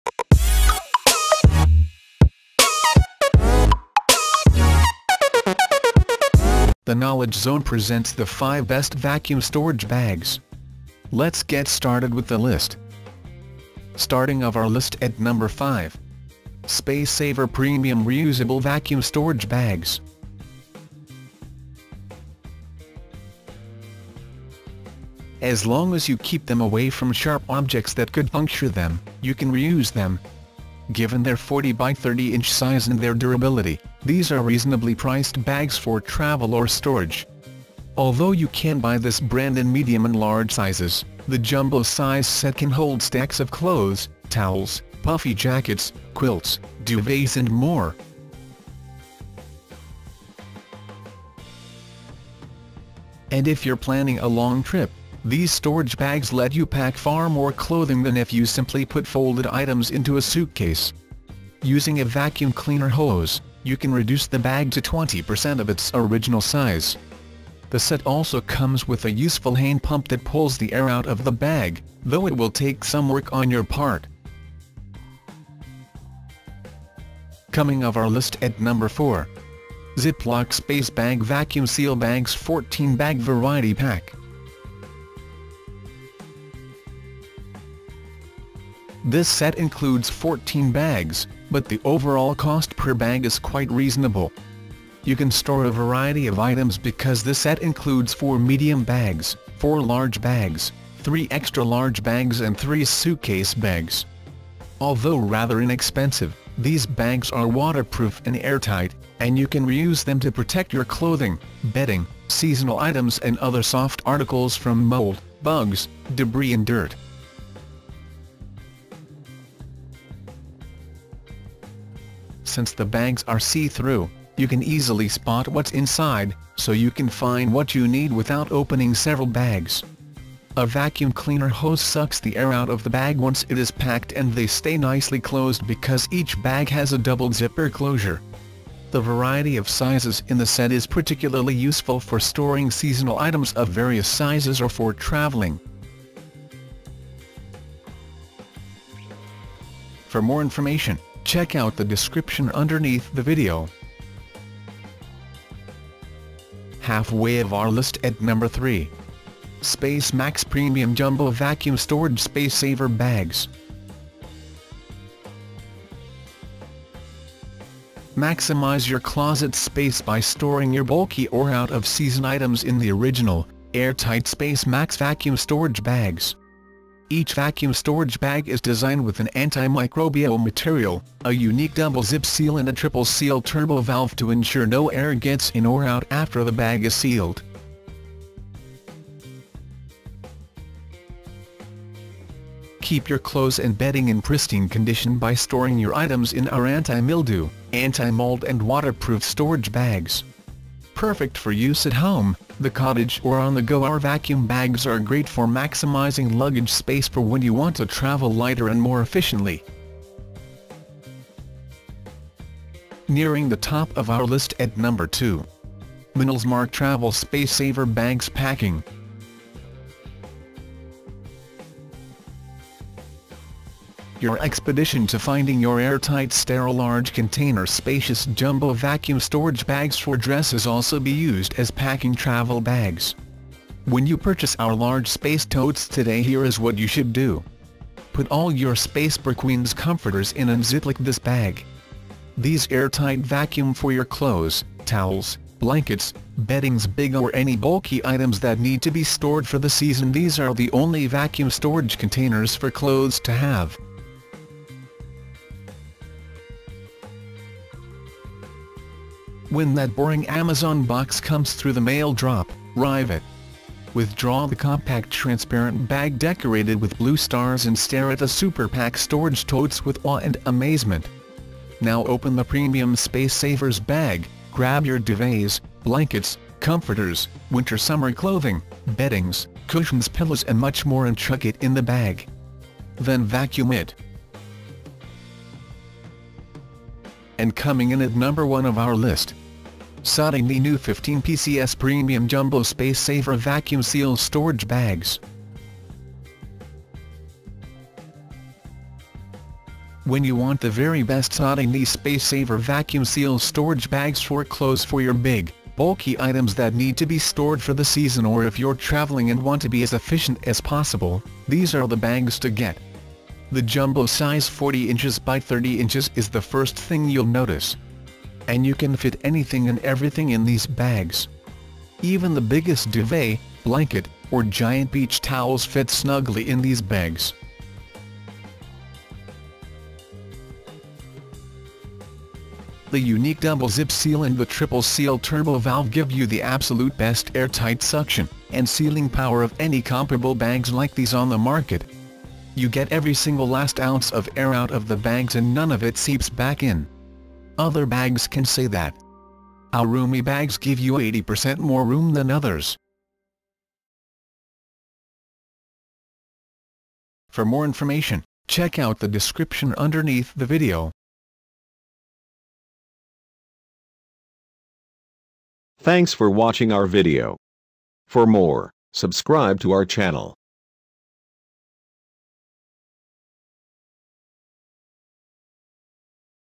Best Vacuum Storage Bags
Hello,
Looking for the detailed review of Best Vacuum Storage Bags? We have tried to show the details in this Review. You can decide which Storage Bags you should buy and which one is good for you, After reading this exclusive review.

Here is the list of 5 Best Vacuum Storage Bags.

01. Sodynee New 15pcs Premium Jumbo Space Saver Vacuum Seal Storage Bags

02. Minals Mark Travel Space Saver Bags Packing

03. SpaceMax Premium Jumbo Vacuum Storage Space Saver Bags

04. Ziploc Space Bag Vacuum Seal Bags 14 Bag Variety Pack

05. SpaceSaver Premium Reusable Vacuum Storage Bags

Thanks!